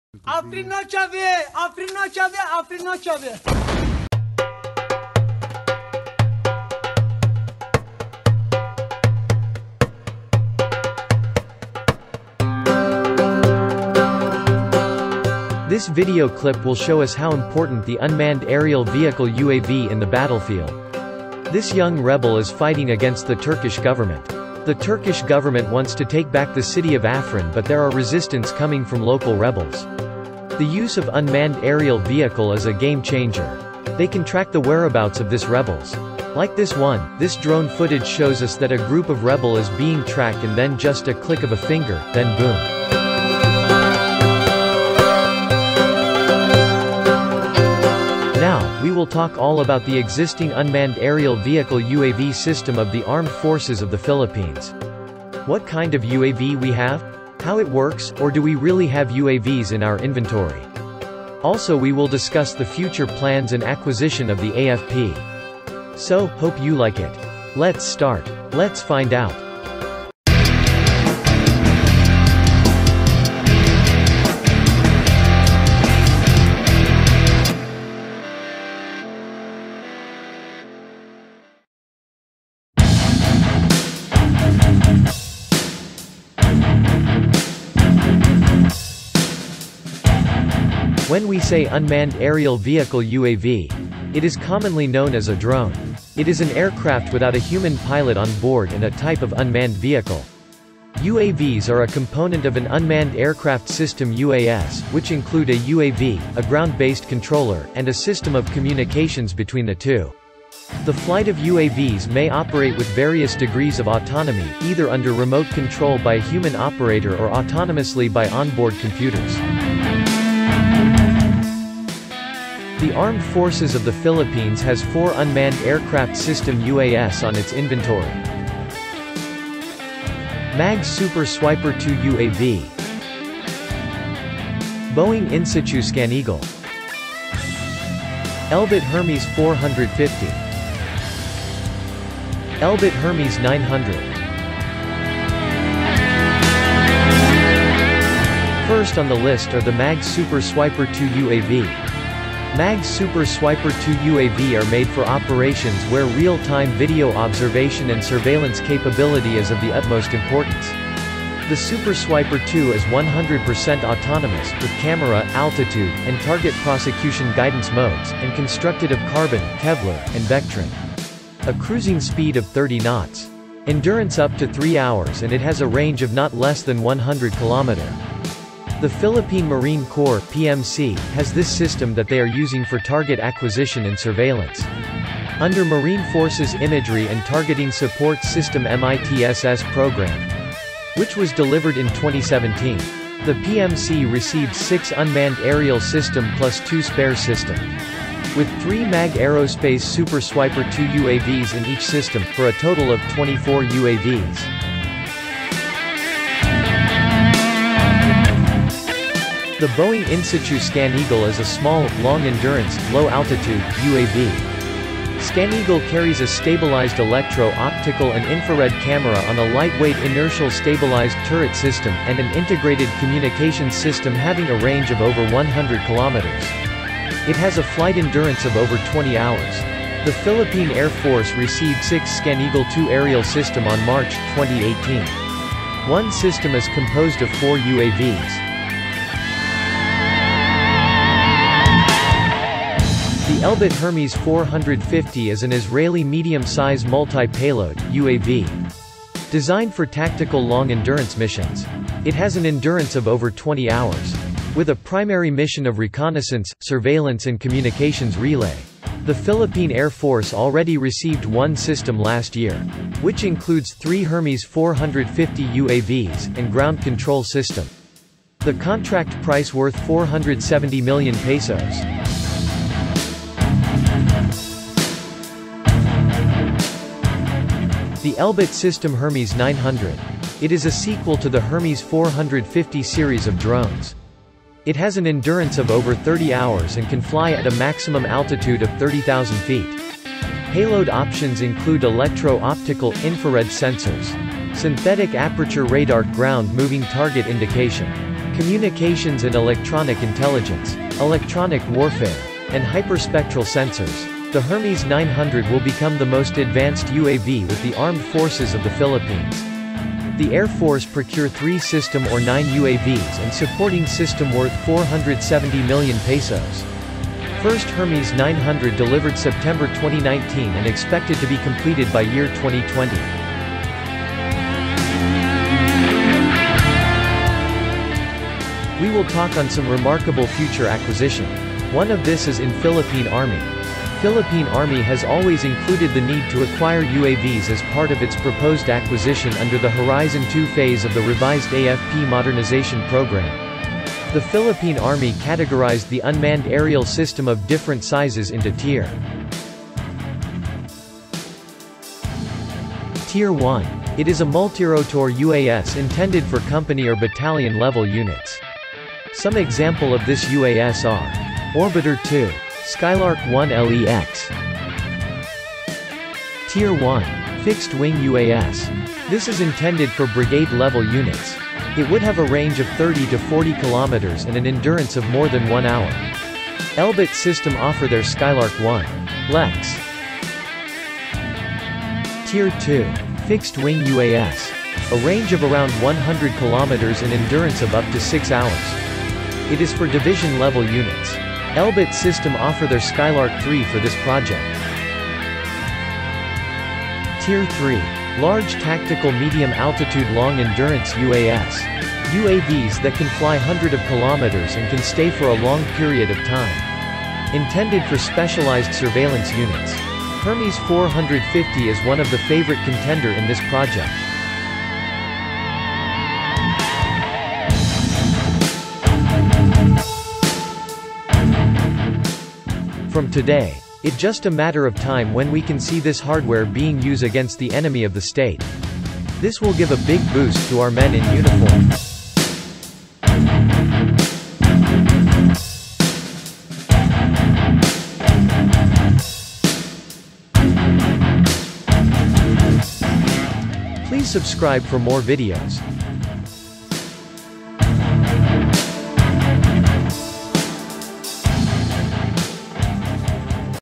List of All Unmanned Aerial Vehicles -UAV's || Armed Forces of the Philippine's
Some of Unmanned Aerial Vehicles-UAV's of the Armed Forces if the Philippines.

An unmanned aerial vehicle (UAV) (or uncrewed aerial vehicle, commonly known as a drone) is an aircraft without a human pilot on board and a type of unmanned vehicle. UAVs are a component of an unmanned aircraft system (UAS); which include a UAV, a ground-based controller, and a system of communications between the two. The flight of UAVs may operate with various degrees of autonomy: either under remote control by a human operator or autonomously by onboard computers.